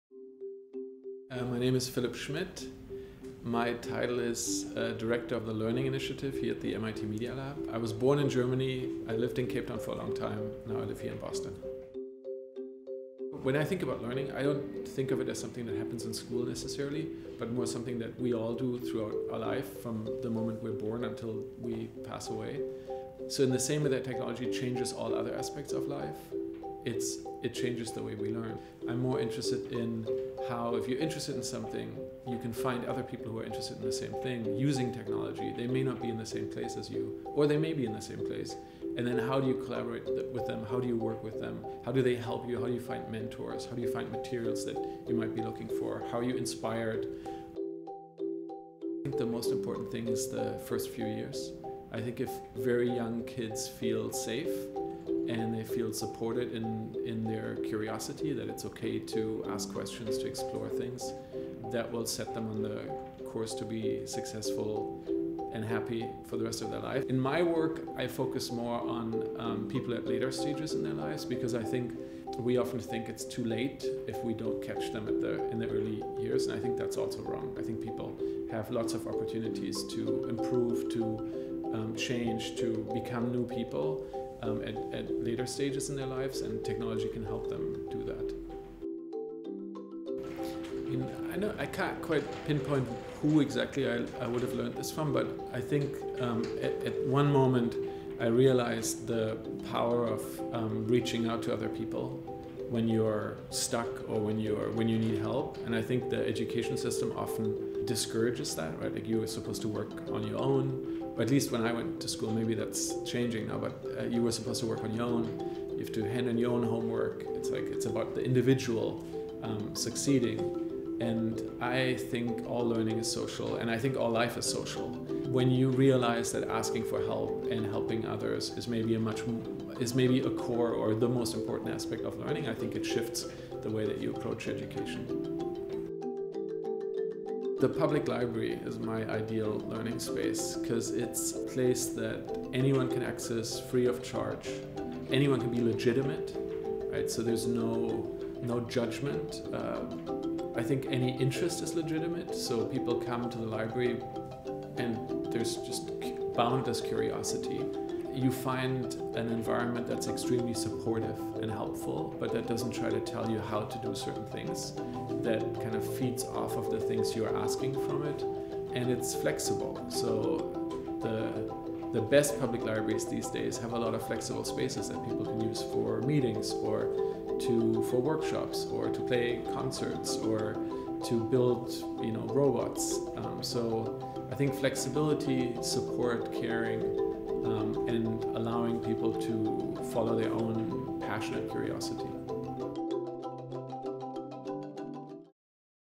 Philipp Schmidt
Philipp Schmidt is principal investigator (PI) and director of the Media Lab Learning Initiative, which studies and promotes the Media Lab’s culture of creative learning.

He is a co-founder and board member of Peer 2 Peer University (P2PU), a nonprofit that provides access to online higher education through public libraries. His work on digital credential systems includes the original concept for open badges and the first prototypes for Blockcerts, an open standard for issuing, managing, and verifying academic certificates using blockchains. Philipp serves on the board of College Unbound, was a founding board member of the Open Course Ware Consortium, and co-authored the Cape Town Open Education Declaration. He has received Shuttleworth and Ashoka fellowships.